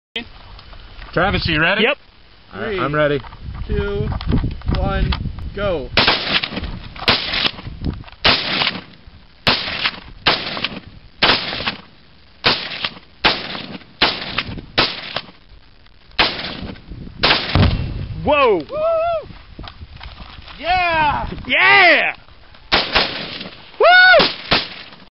2lbs of tannerite in a microwave at slab city
2lbs of tannerite in a microwave with 1 gallon of gasoline at Slab City